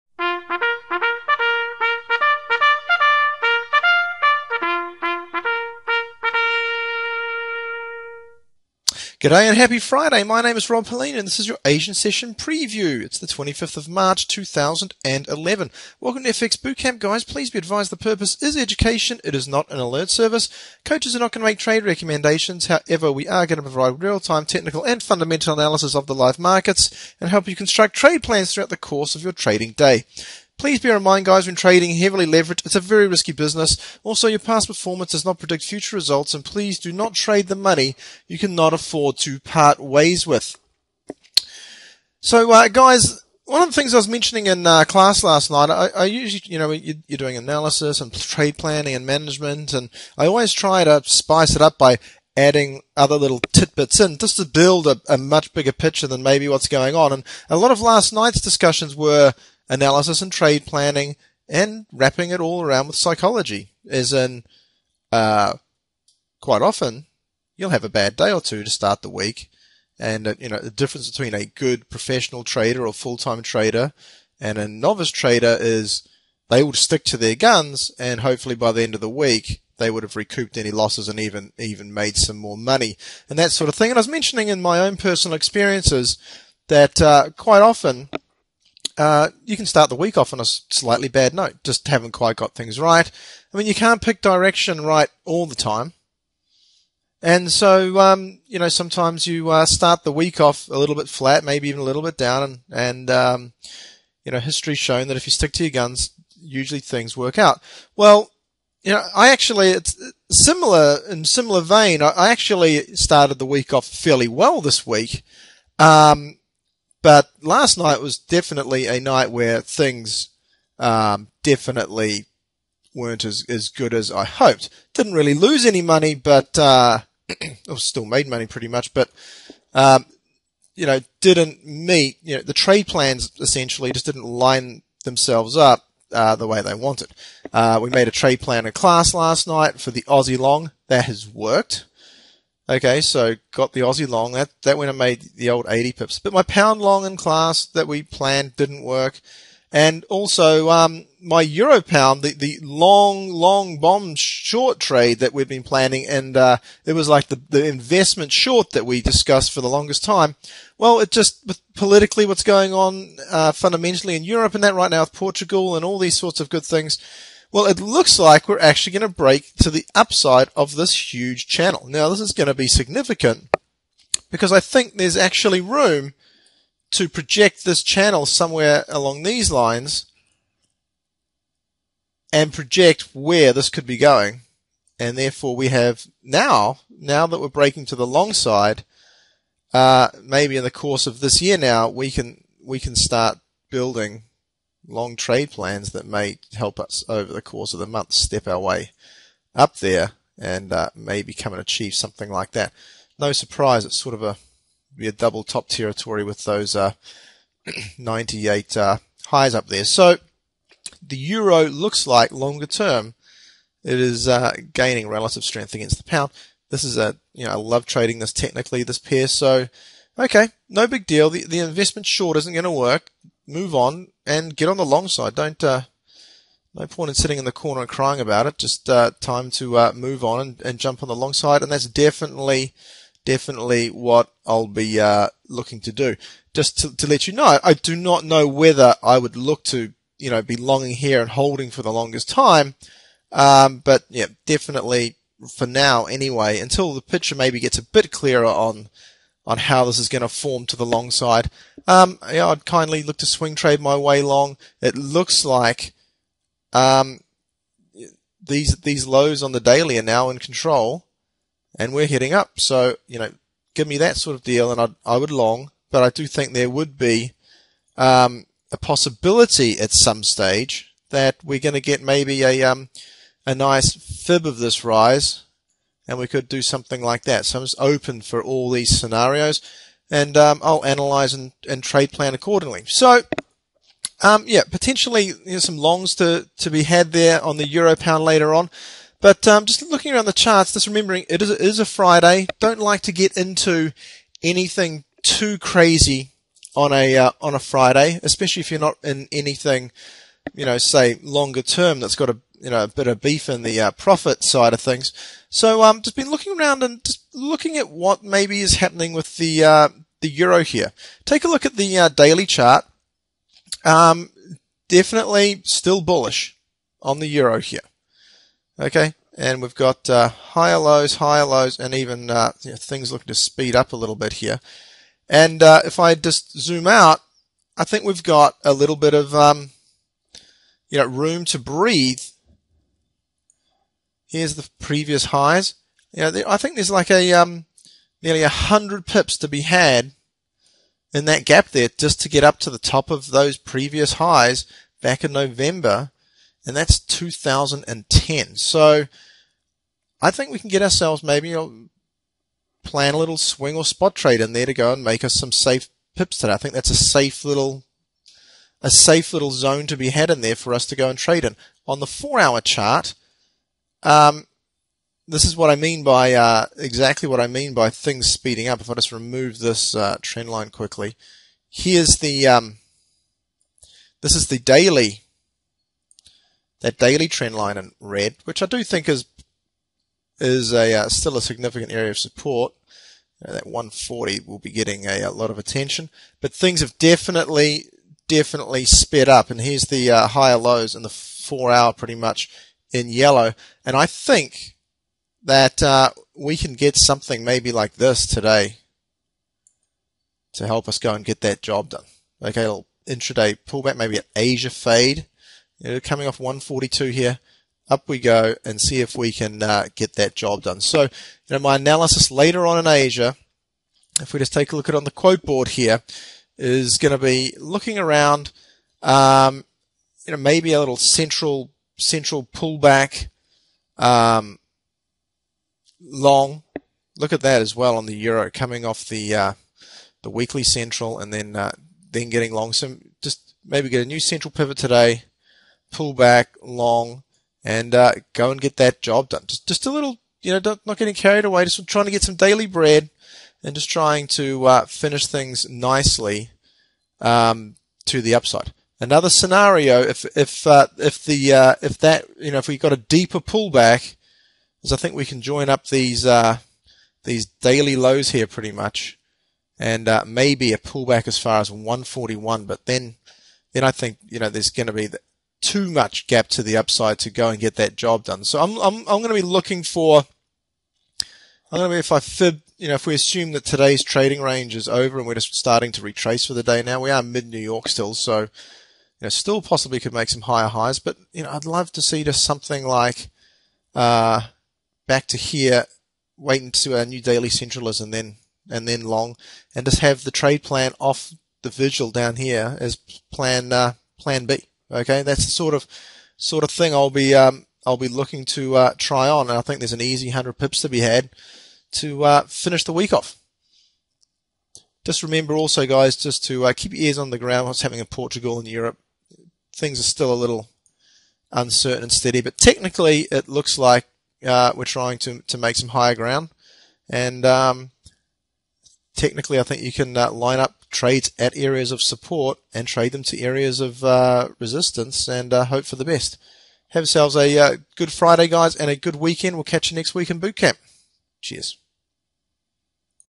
FOREX VIDEO - ASIAN OUTLOOK MARCH 25 2011
Happy Friday

For todays video, I am looking for what I think will be a safe trade to get us in to the weekend. Not looking for anything spectacular, just something to give us daily bread and a smooth run in to the weekend.

Take care folks

Regards

Rob Helean